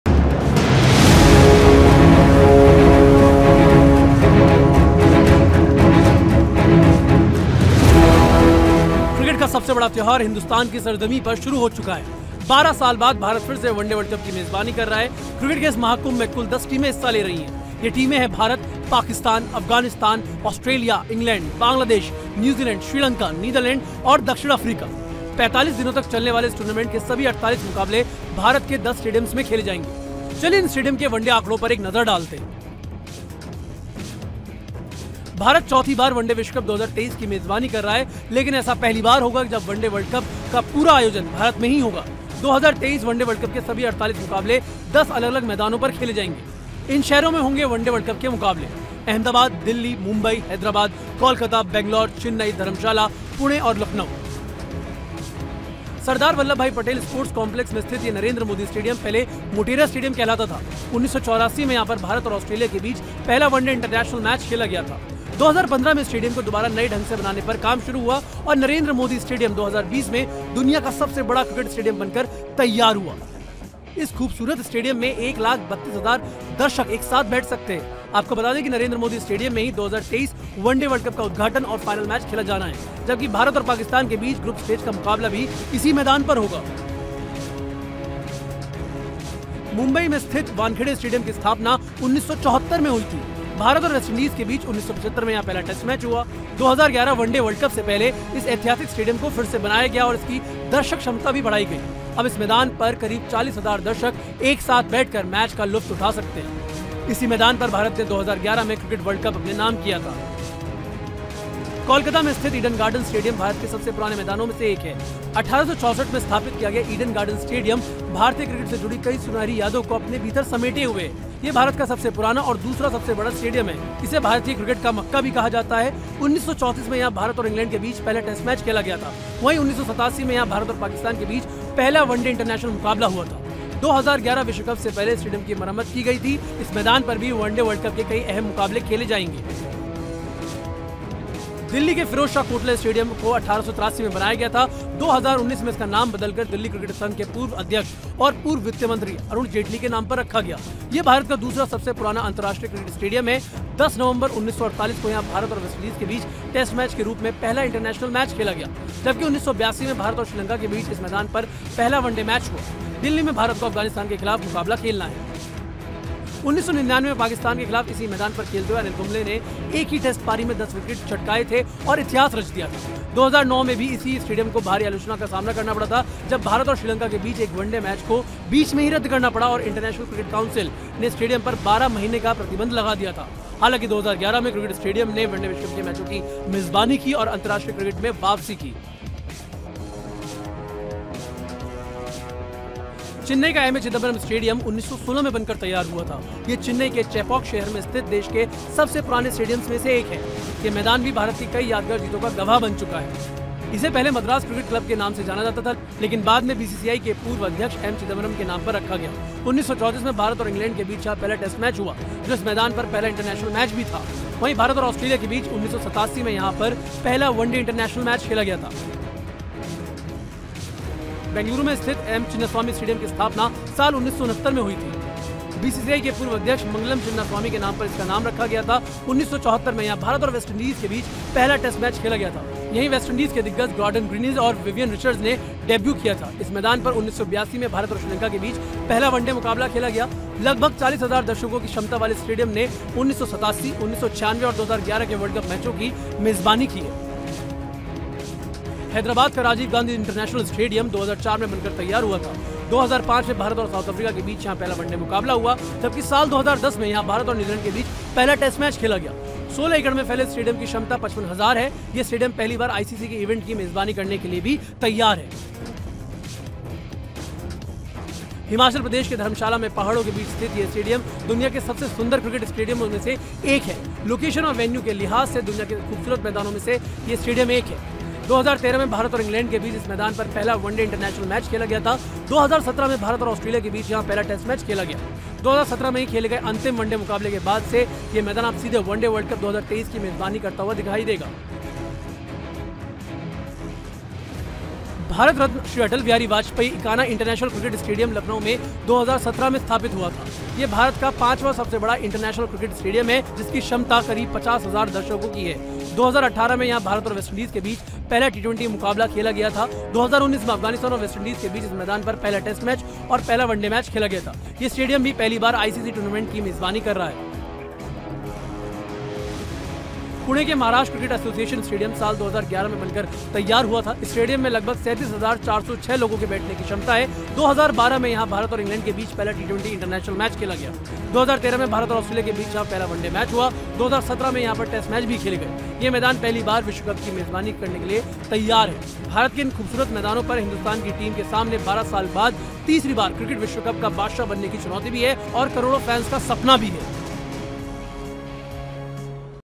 ODI WC 2023 : India में हो रहे वनडे वर्ल्ड कप में कैसे दिख रहा है 10 का दम,देखें वीडियो | Team India
ODI World Cup इस वक्त India में खेला जा रहा है, आइए आपको बताते हैं की जिन मैदानों पर ये मुकाबले खेले जा रहे है कैसा है उनका इतिहास। पूरी लिस्ट जानने के लिए देखें ये वीडियो।